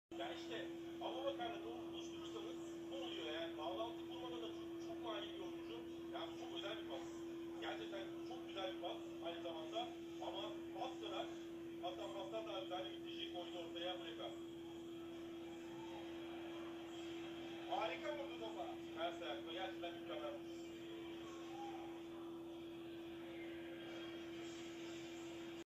JACKSON MULEKA 2-0 BEŞİKTAS-TİRANA
jackson muleke durumu 2-0 yaptı beşiktas gol atmaya devam ediyor .